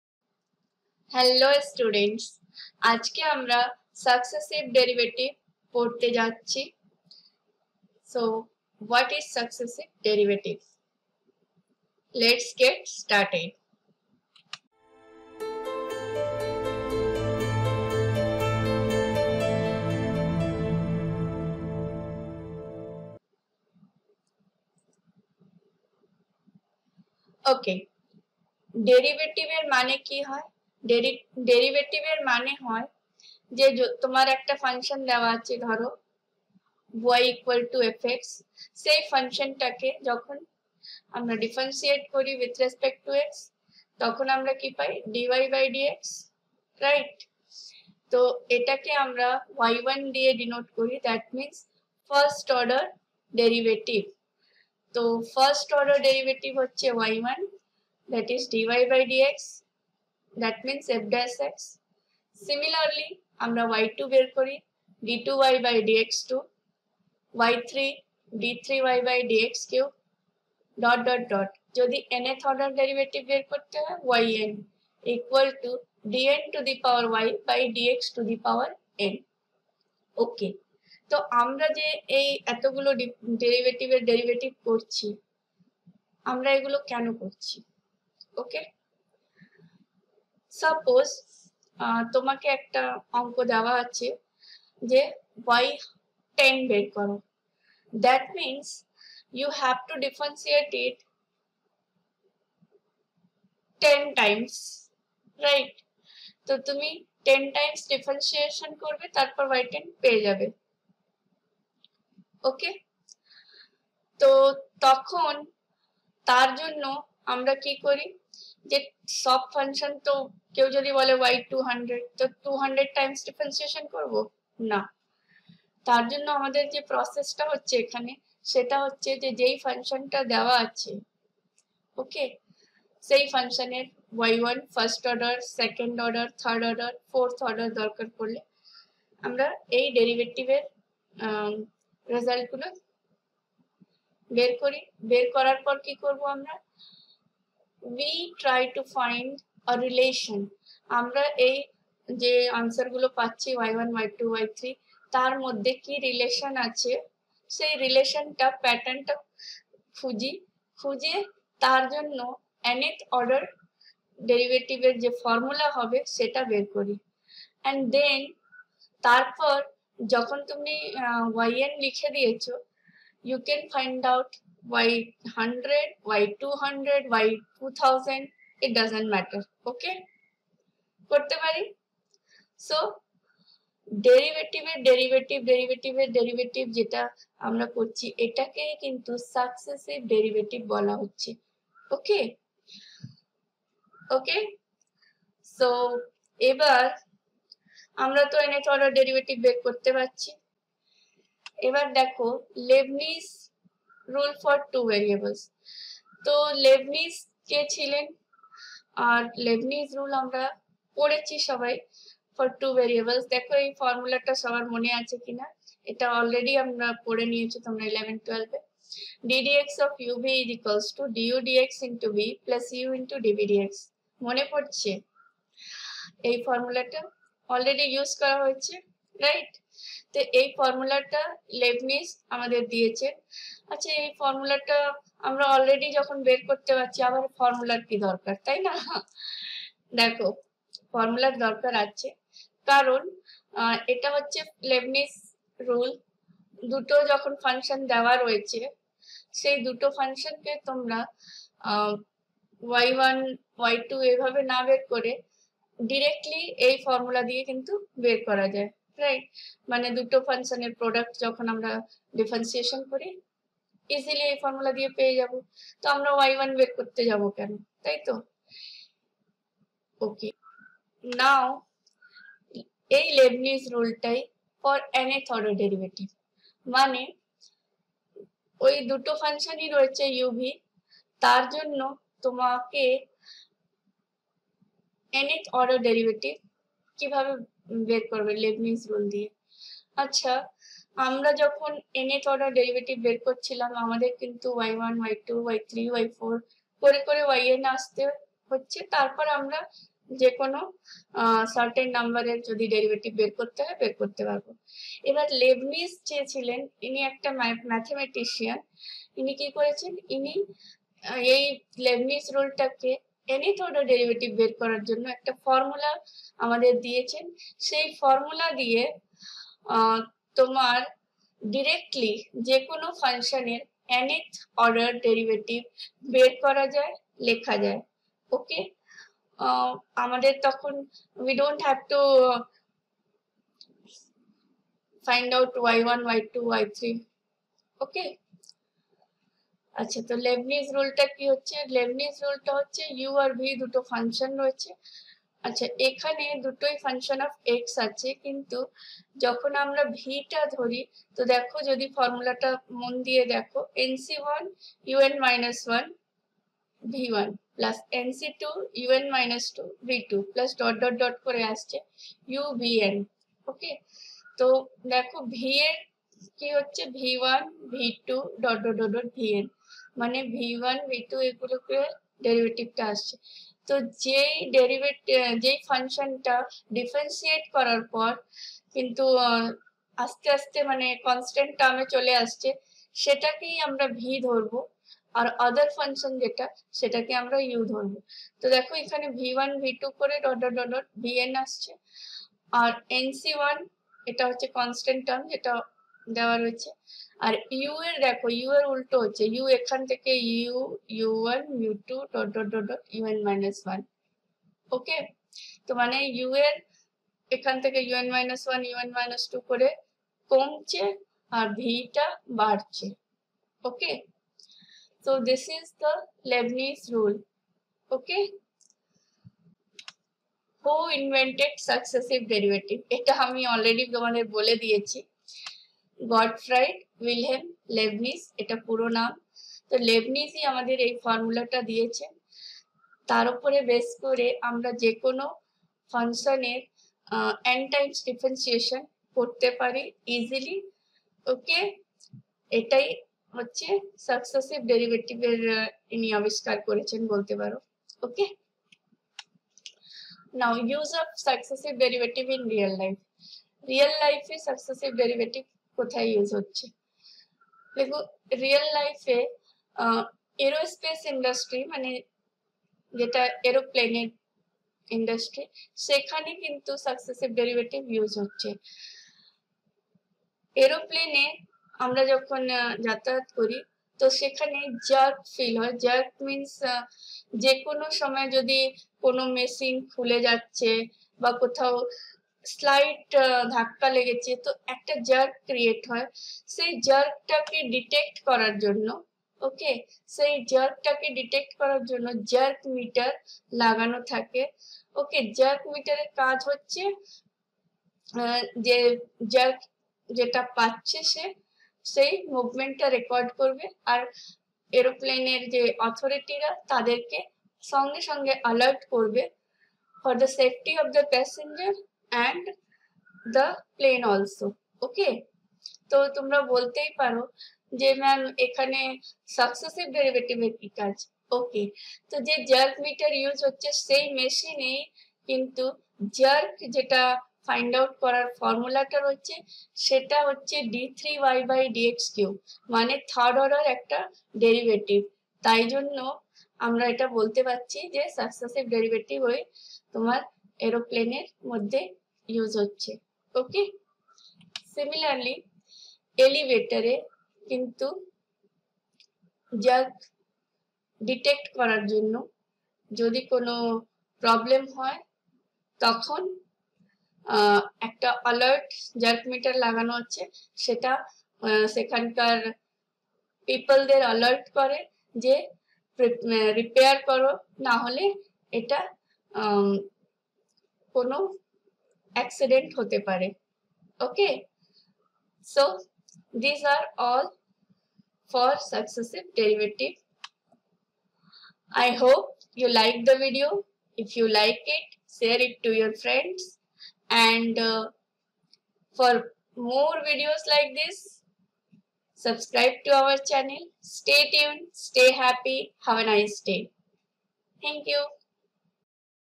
Use of Successive Derivative in real life, Leibniz rule for nth order derivative, Bsc Pass & Honours
Definition of Successive Derivative, Leibnitz Rule for nth order derivative ,
Use of Successive Derivative in real life.
Bsc maths first Semester  , Differential Calculus

Bsc Maths General Sem-1  CC1  year 2022,  BSc Maths Pass Sem -1  CC1  year 2022

BSc Maths Honours Sem-1 for year 2022 Calcutta University